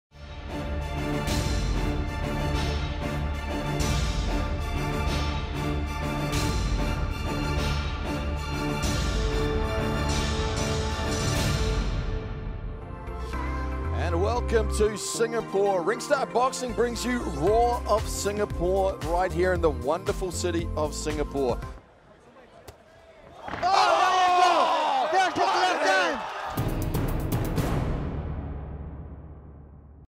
Michael "Gloves on Fire" Dasmarinas!
Michael Dasmarinas Blasts Out Karim Guerfi For IBO Strap.. RoarofSG5 Coming Soon.